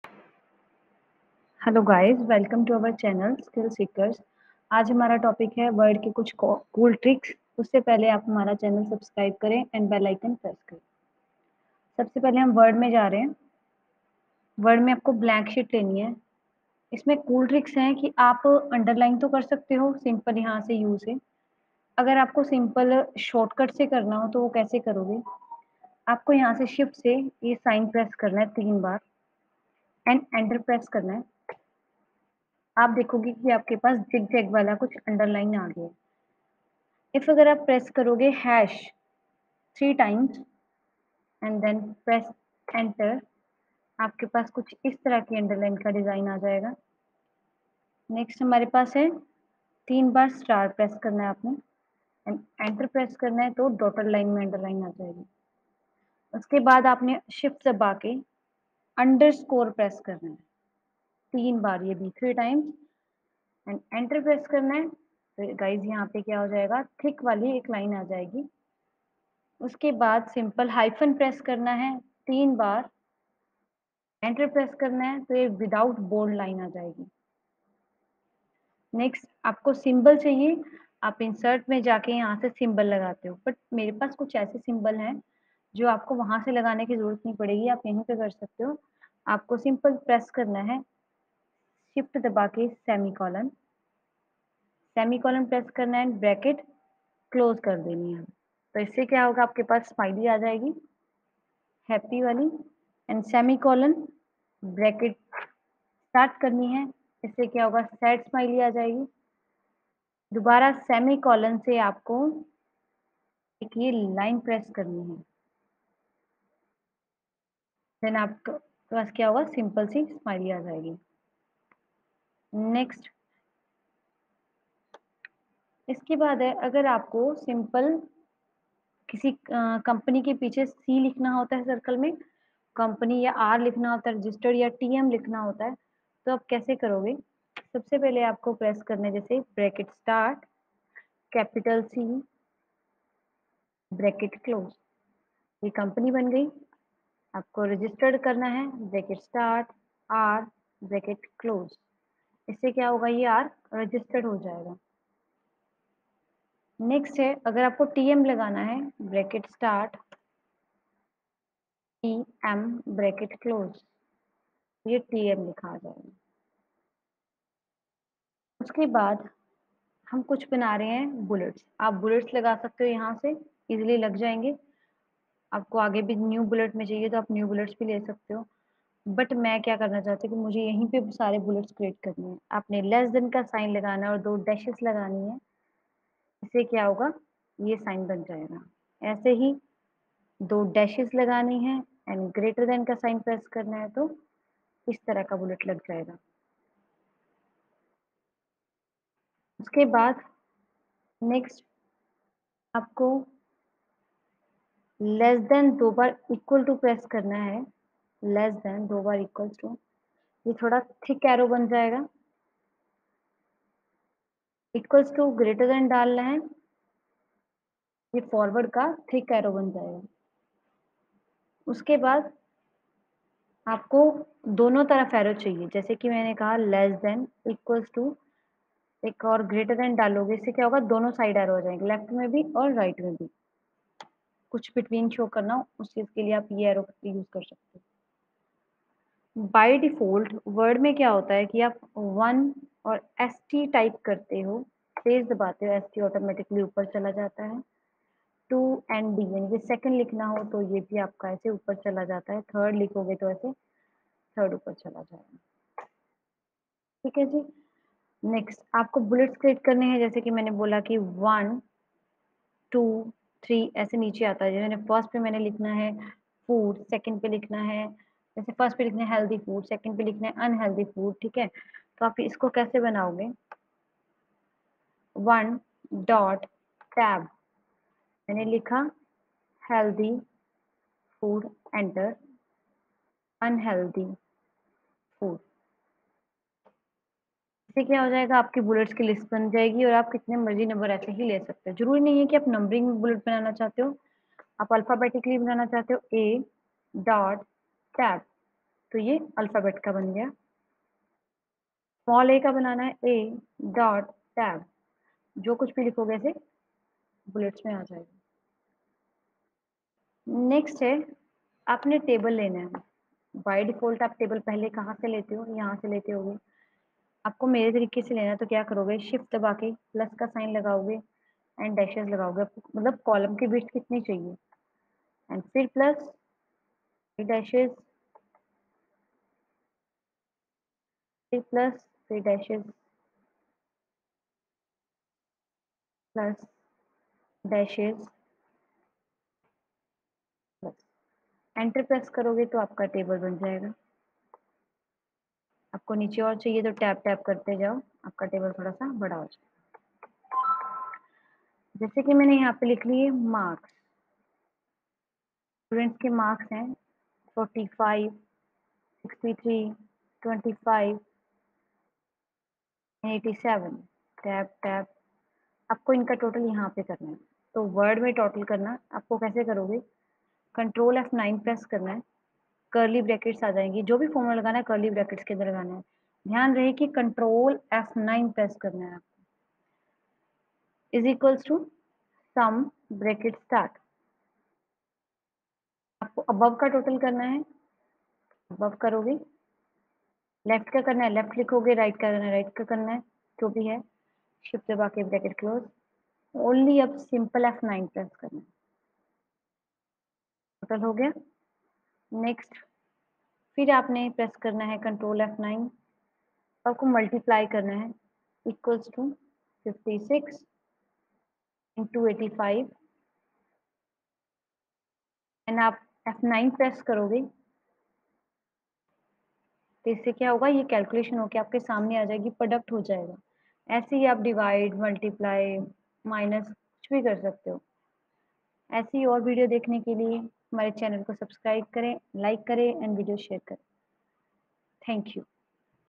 Cool Tricks in Microsoft Word | Easy and Useful tricks
In this video, we will show you some cool tricks in Microsoft word that are very useful.